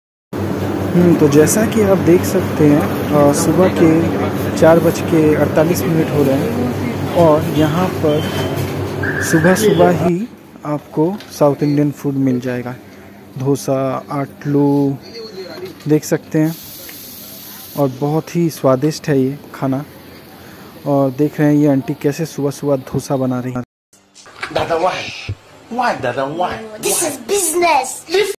Dosas available from 3:30 am everyday in Chennai (Perungudi) Near World Trade Center /
South Indian Street Food @ 3:30 am in most of the areas in Chennai

Location: 16, Vivekananda St, Kandancavadi, Perungudi, Chennai, Tamil Nadu 600096